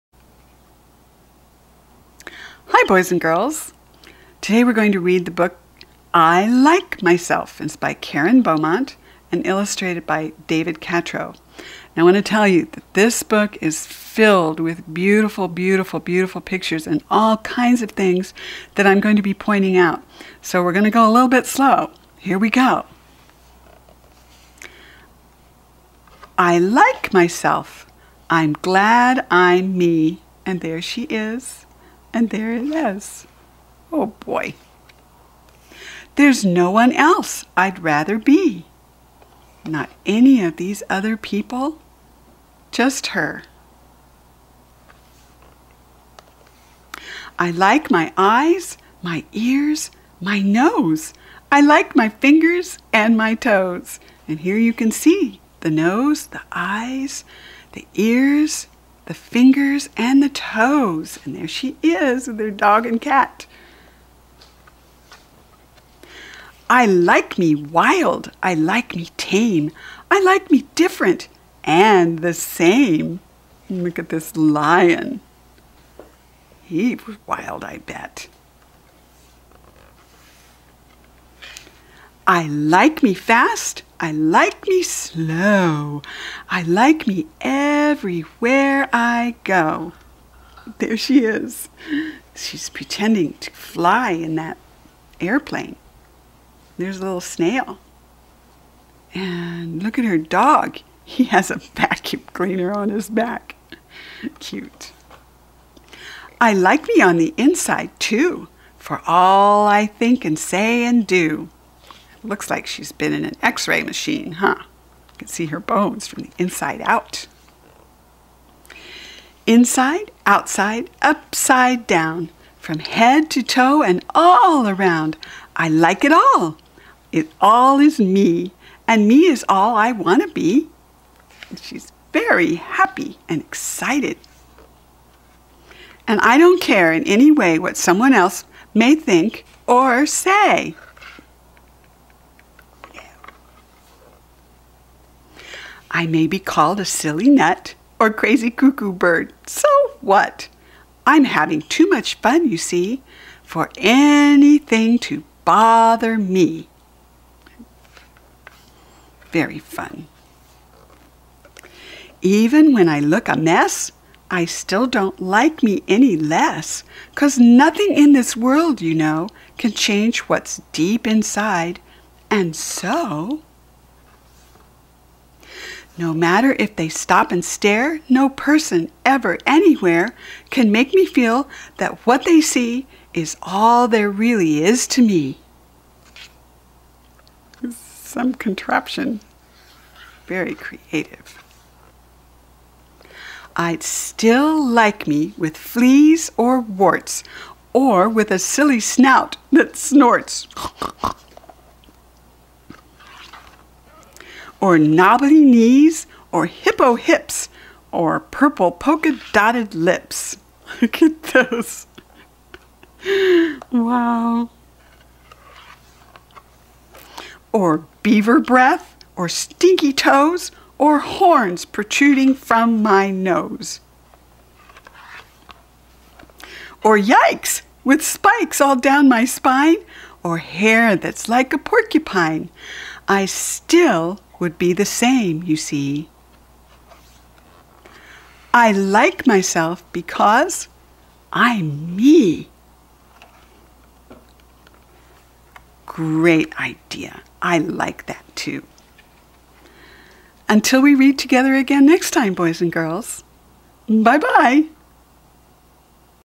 I Like Myself -- Read by Nita
Follow along as Nita teaches you to read while you learn about all the wonderful things you will see!  Learn perfect standard American English. Please click the Thumbs Up - Like button!